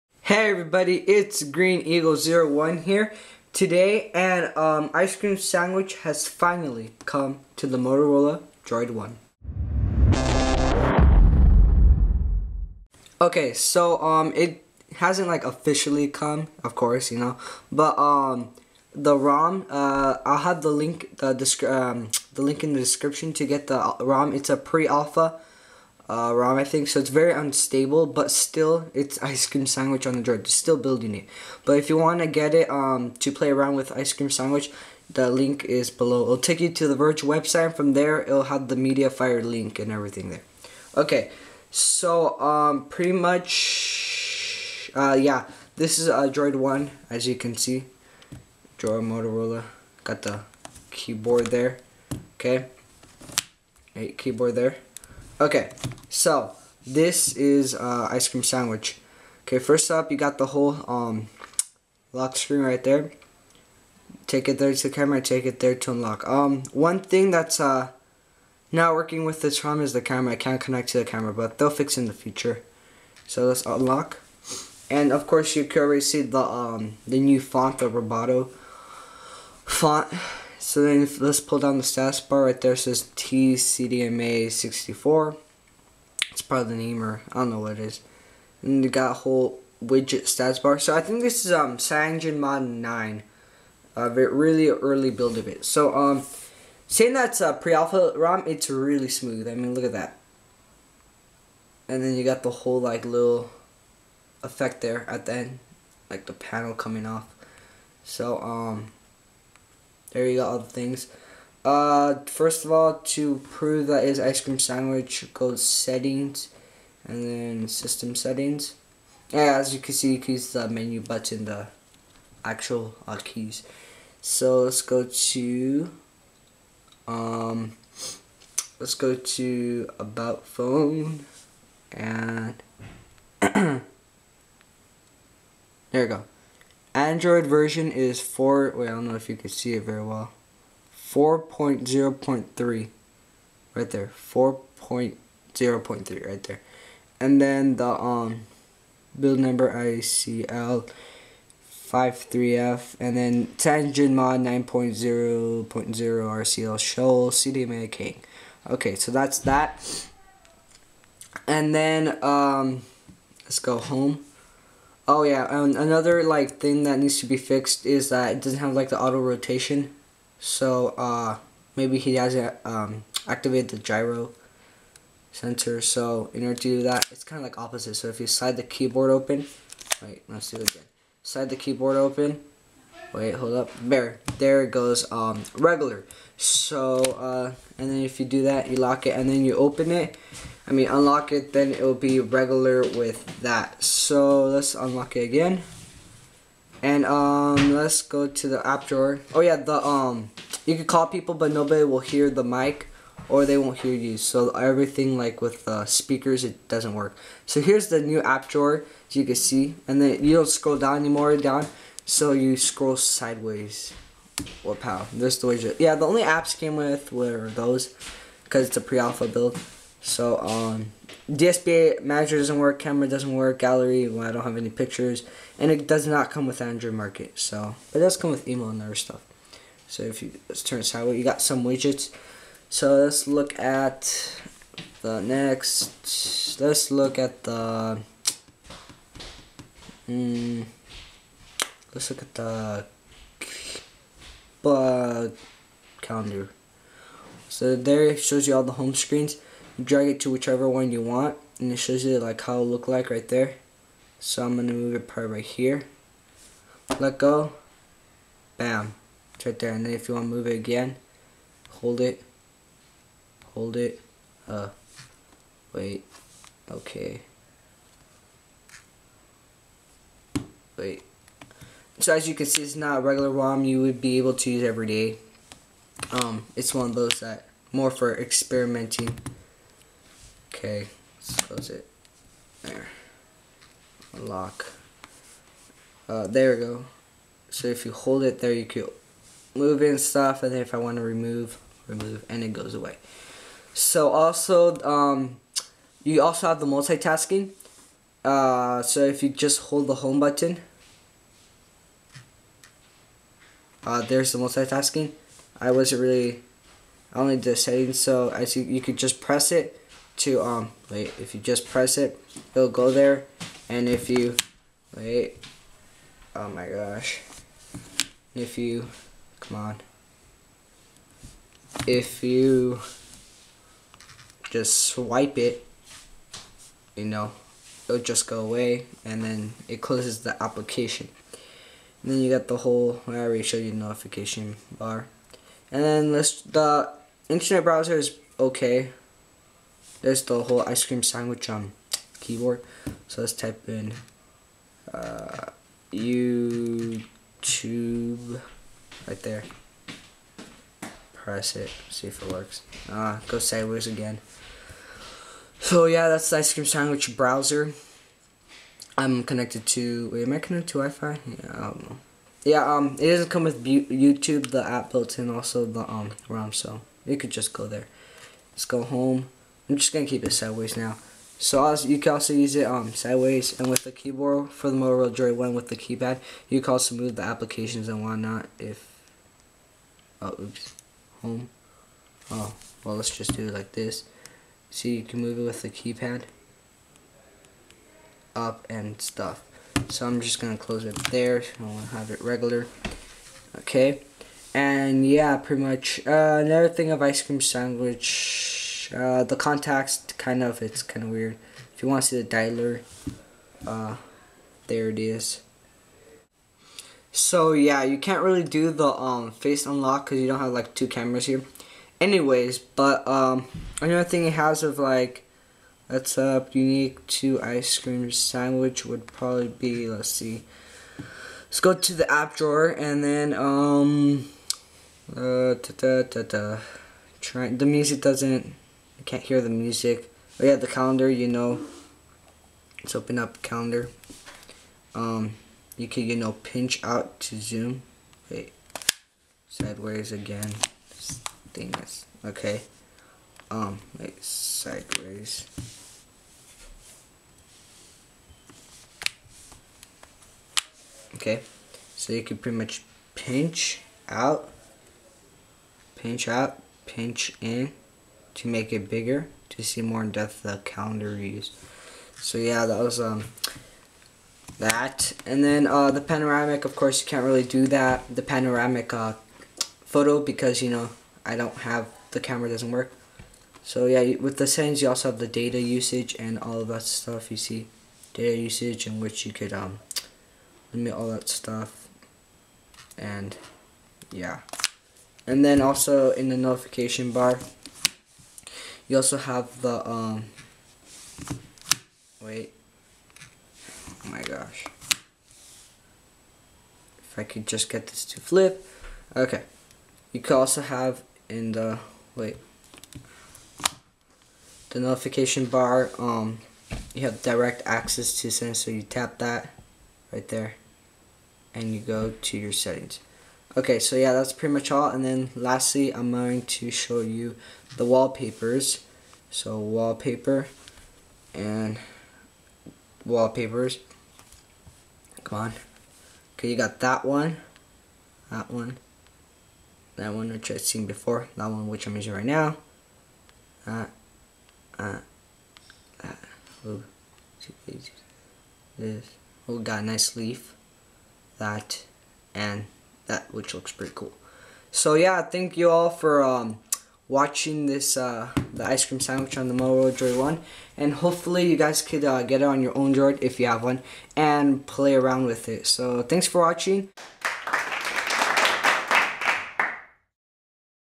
Android Ice Cream Sandwich on Motorola OG Droid 1 HD
This is a review of a pre alpha build of Cyanogenmod 9 (android 4.0) rom on the Motorola Droid 1.